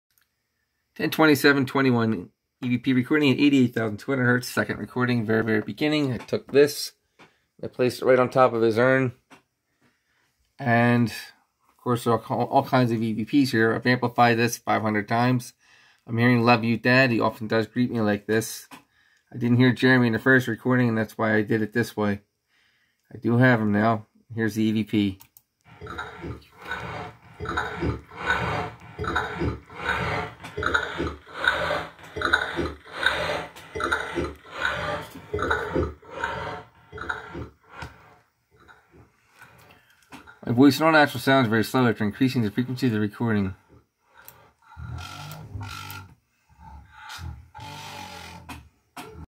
4. My son Jeremy’s EVP “Love you Dad”
I was not sure if I had heard him on the first recording, so I placed one of my recorders on top of his urn and recording this.